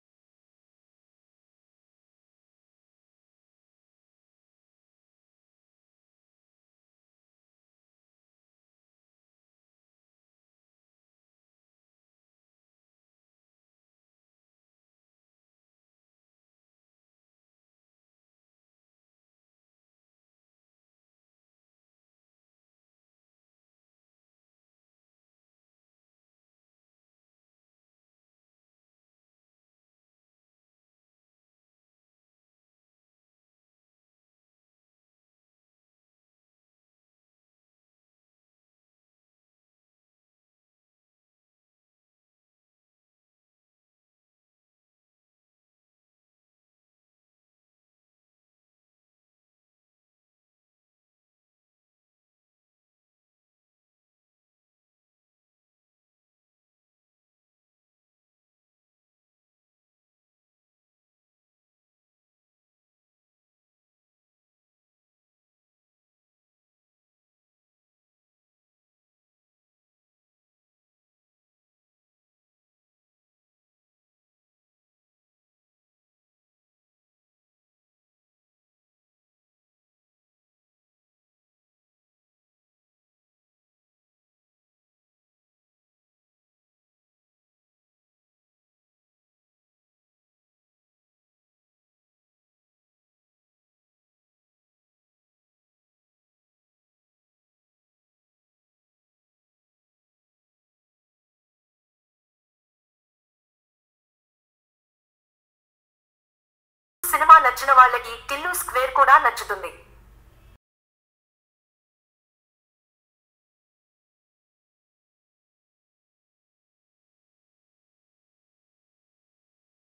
TILLU SQUARE MOVIE REVIEW || SIDDHU JONNALAGADDA || ANUPAMA PARAMESWARAN || DJ TILLU
After the grand success of DJ TILLU, SIDDHU JONNALAGADDA is back with TILLU SQUARE but this time with LILLY as his love interest. Watch the video to know how the movie was.